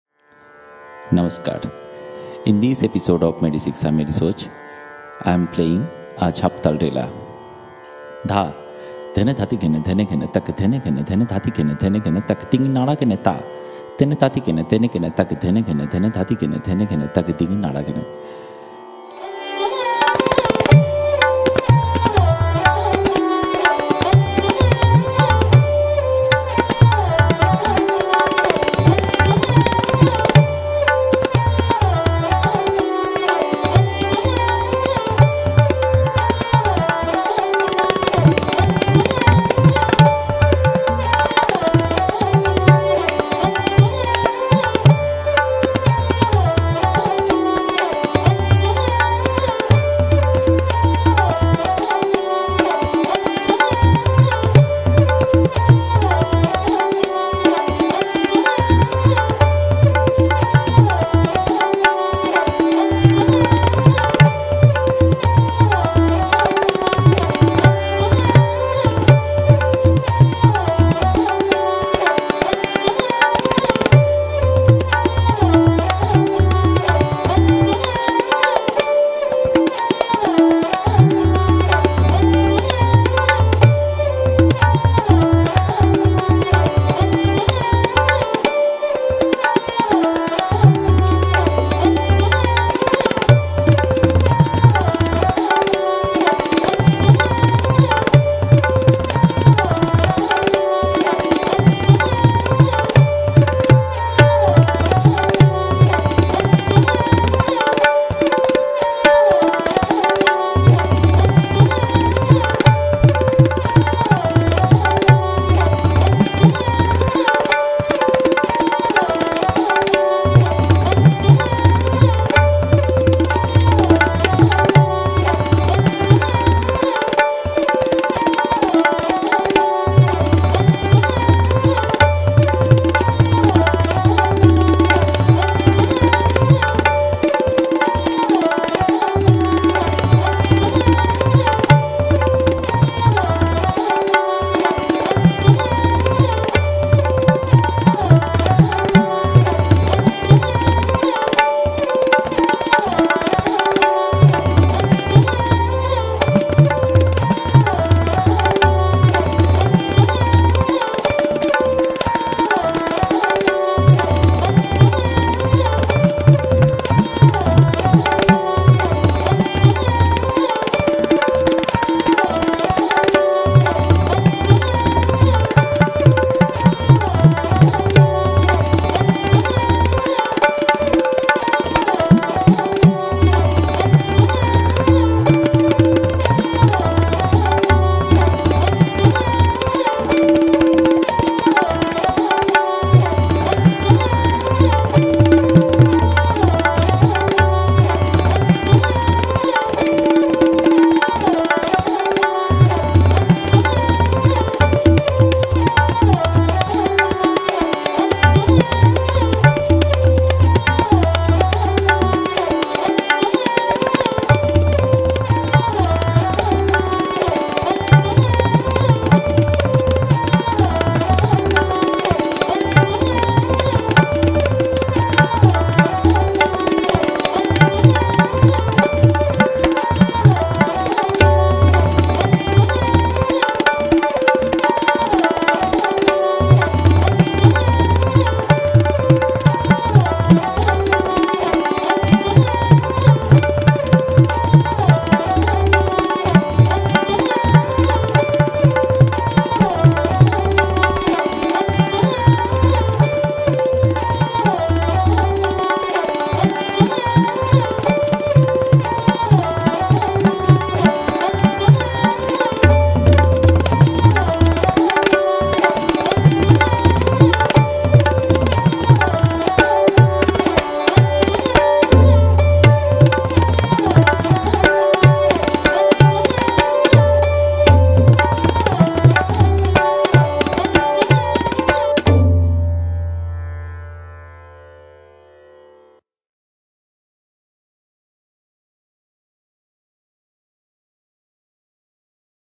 Virtuosic Rela in Jhaptaal || Emon Sarkar || Meri SIKSHA Meri SOCH - EP 44
In this episode, listen to this wonderful Rou in Jhaptaal (10 beats)

Here's our forty fourth of 'Meri SIKSHA Meri SOCH" - A Tabla series by Emon.

We are Satrangee - the classical quartet working to spread Indian Classical Music.